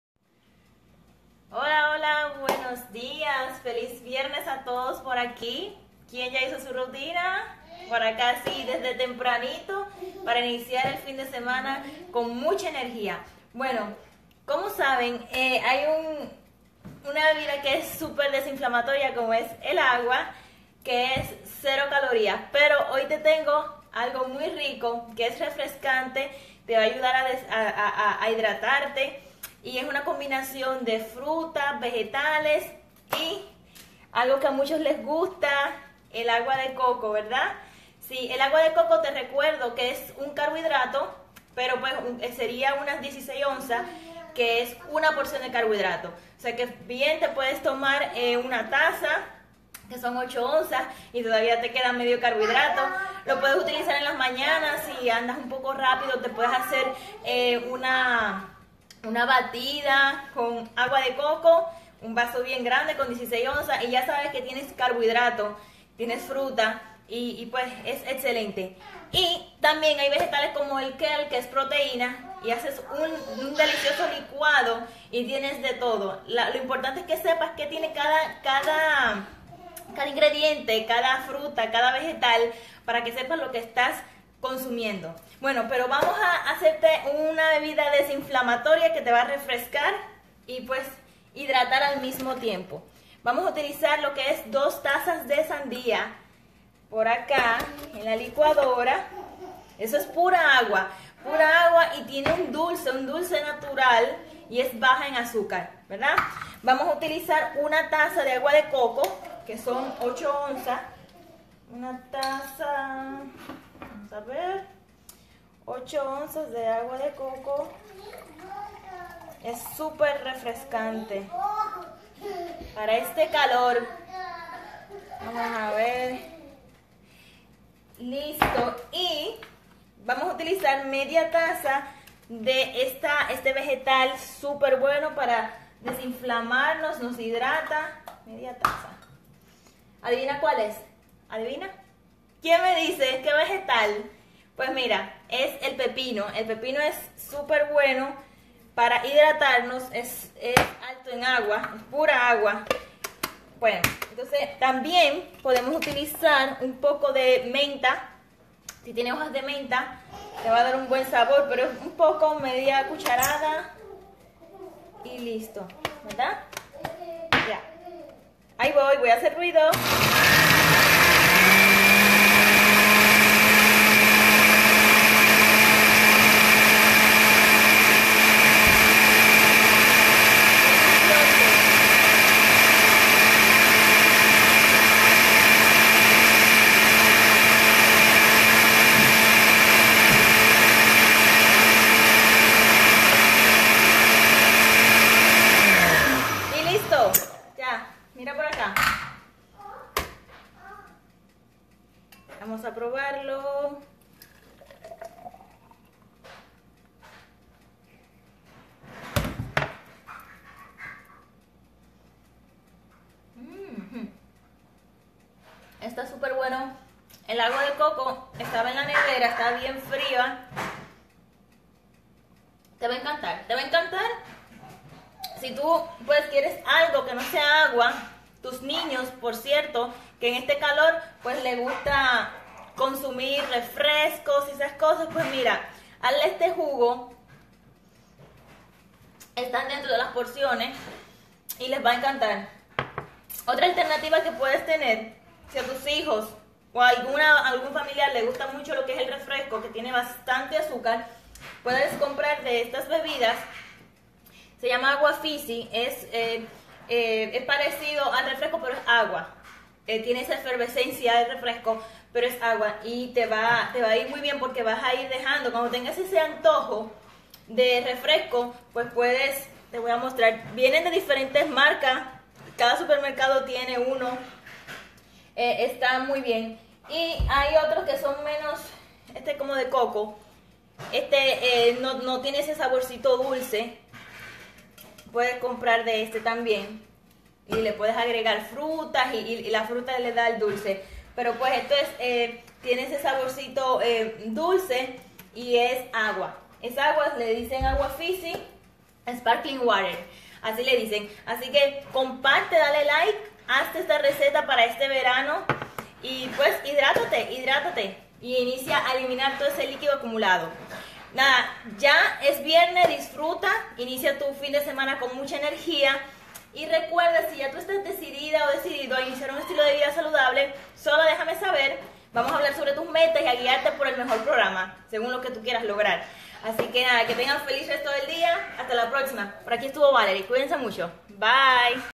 Jugo de Sandia Anti Inflamatorio
Prepara esta rica receta de jugo de sandía, hoy. El agua de coco y la sandía son poderosos diuréticos que también te hidratarán.